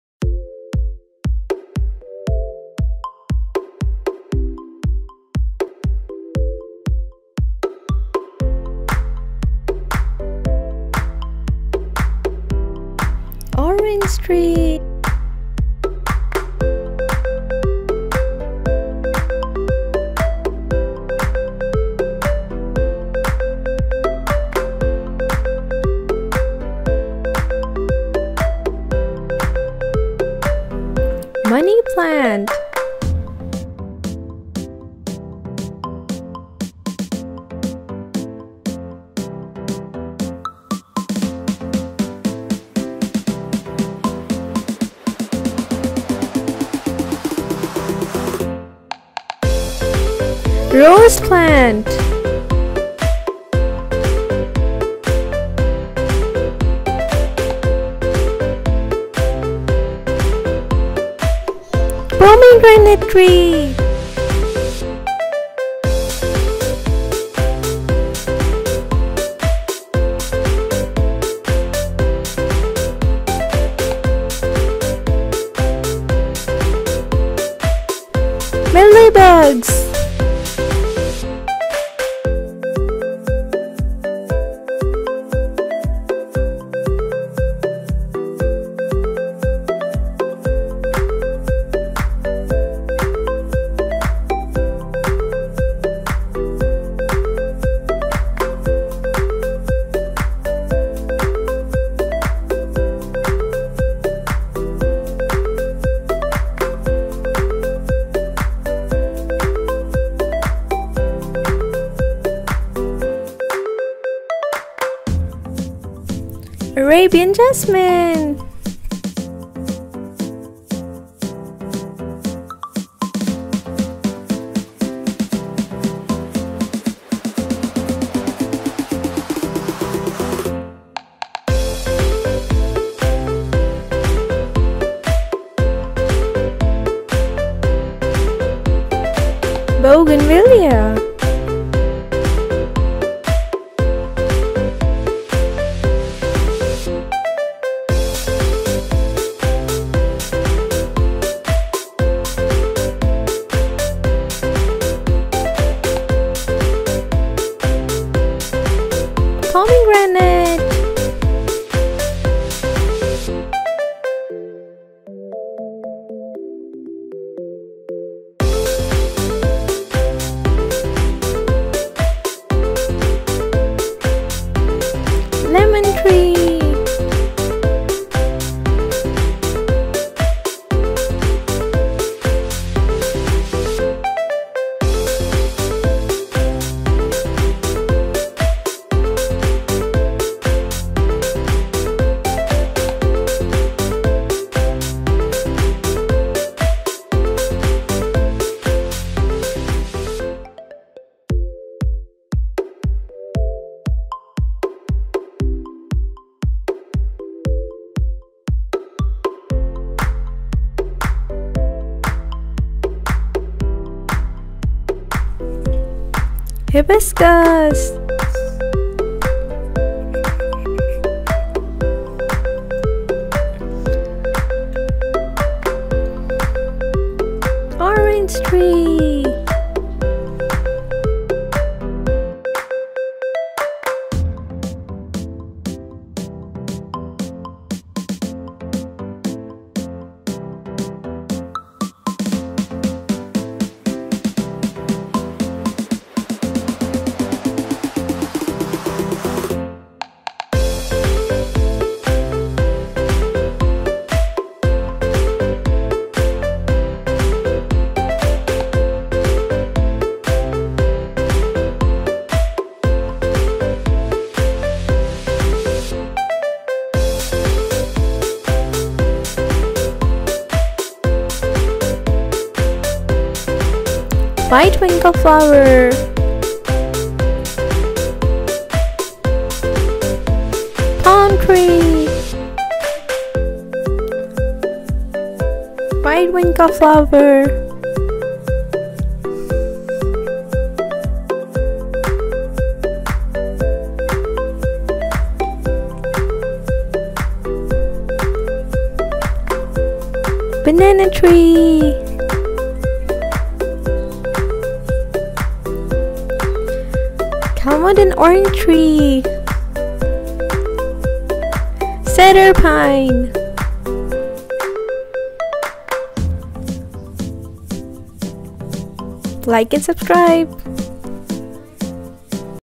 LEARN PLANT NAMES FOR KIDS!
David learns about various plants in  his garden!